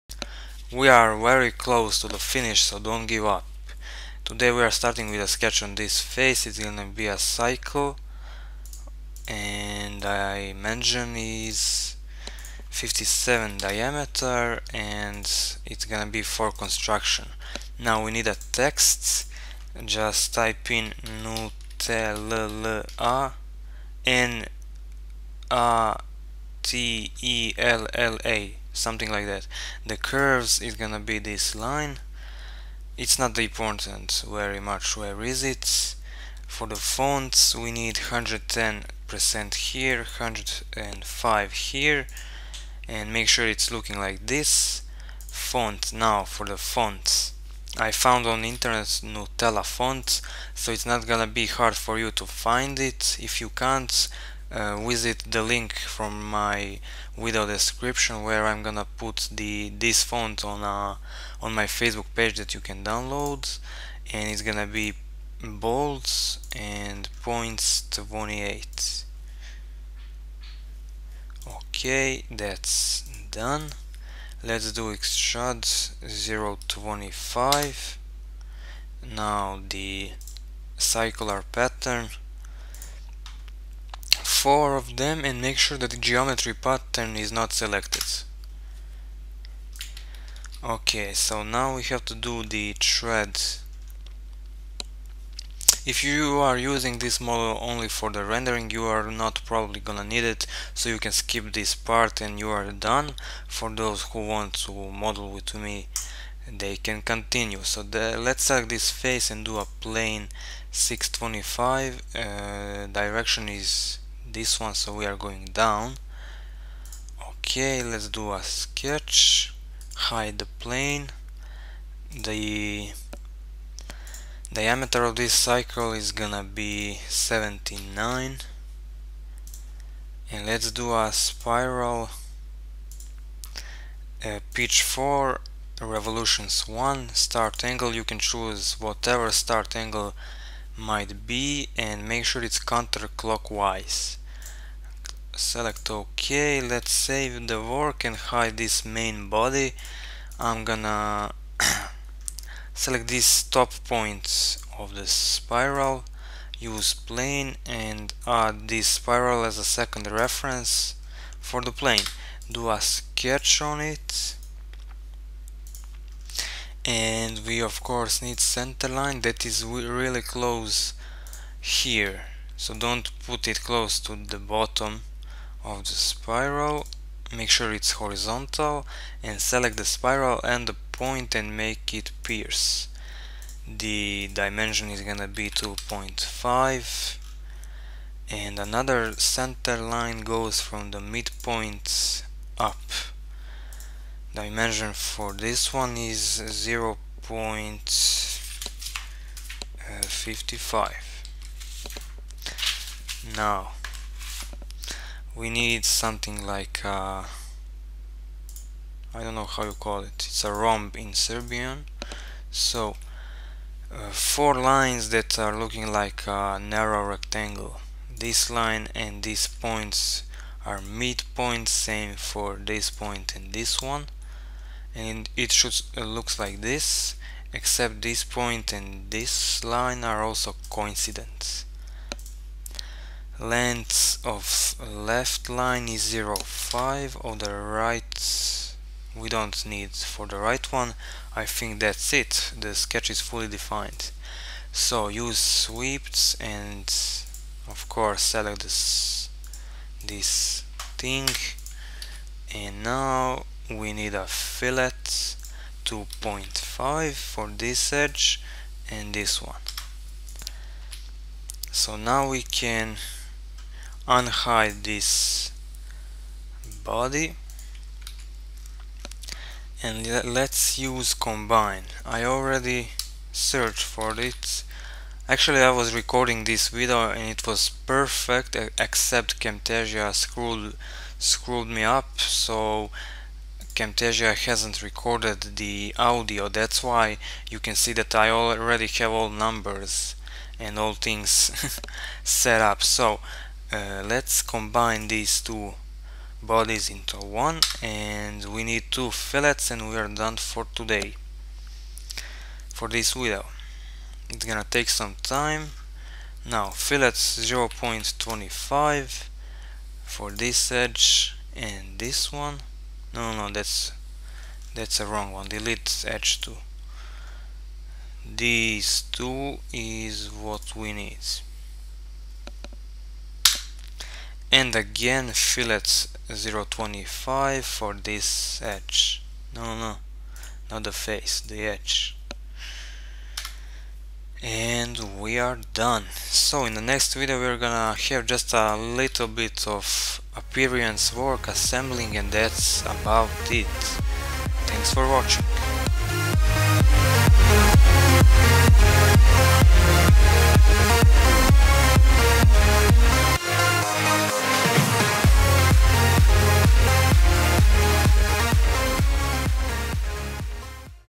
Nutella jar - SolidWorks surfacing tutorial #8
Nutella jar - SolidWorks surfacing tutorial part8

The lid is finished in this video and in the next one we got left to put the cream inside of the bottle and prepare model for rendering, we are almost done!